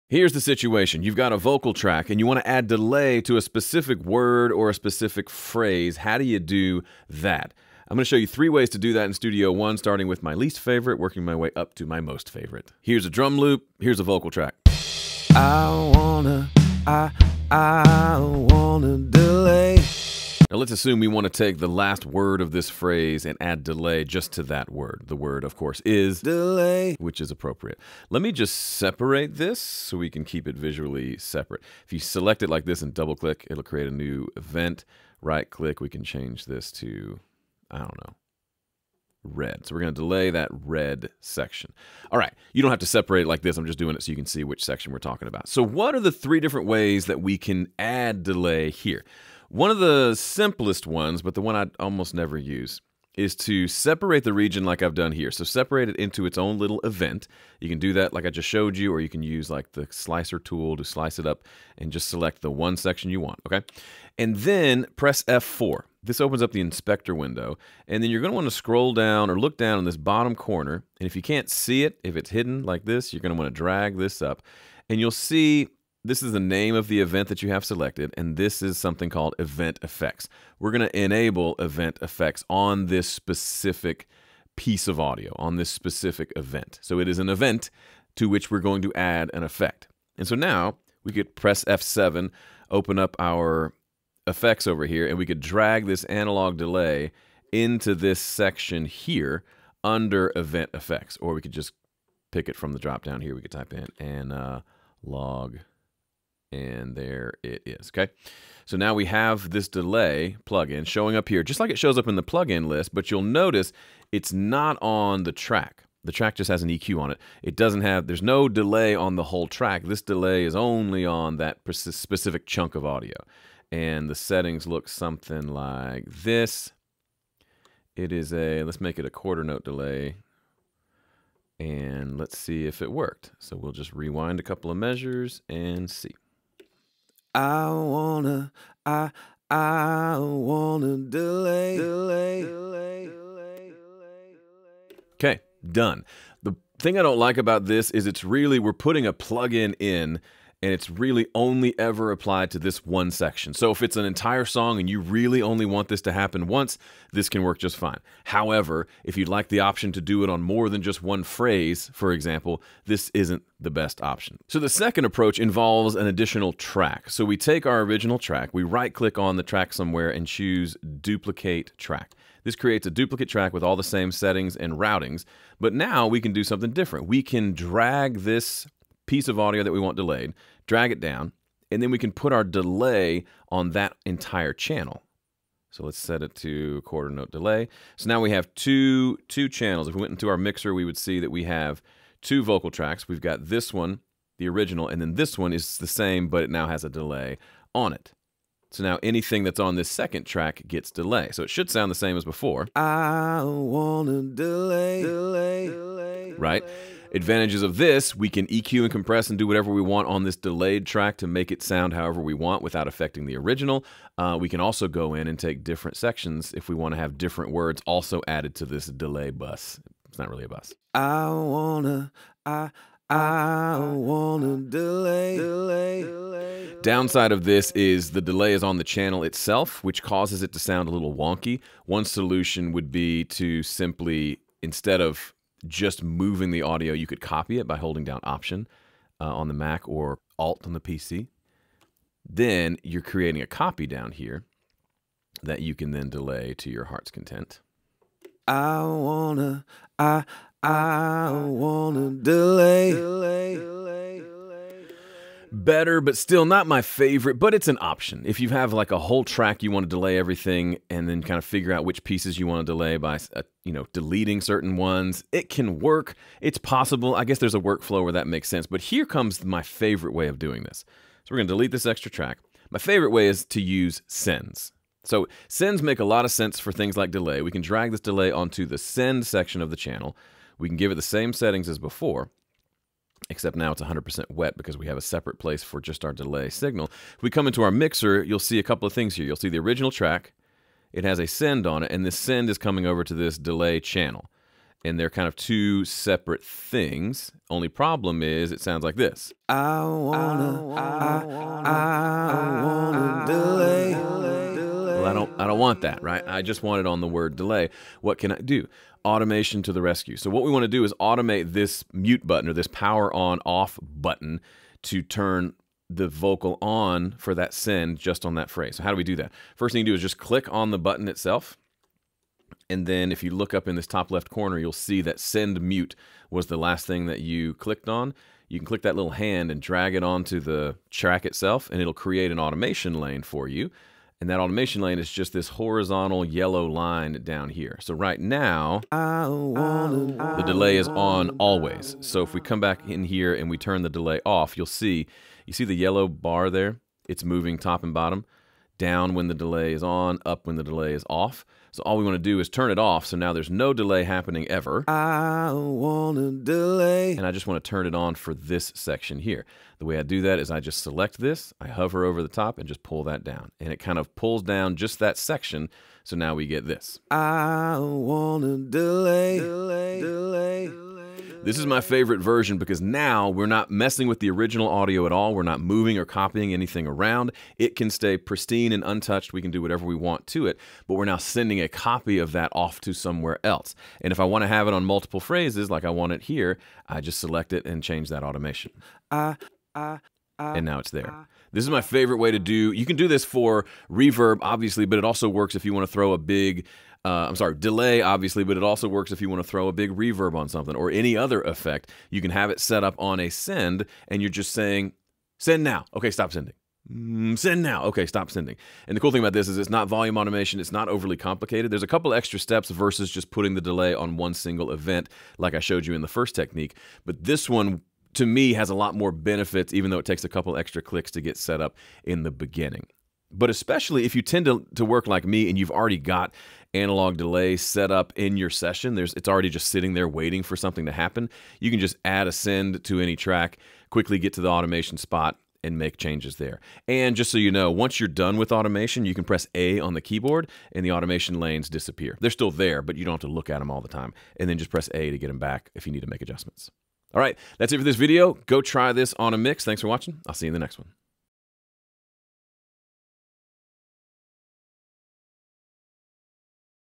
How to Add Effects to a Specific Vocal Phrase | PreSonus
Time-based effects like reverb and delay can add depth to your audio elements and improve your mix. However, what if you prefer to only hear an effect on a single guitar note, or a specific phrase on a vocal take?

In this tutorial, Joe Gilder demonstrates 3 different ways you can use effects precisely on specific moments in your Studio One sessions. Which one of these methods is your favorite?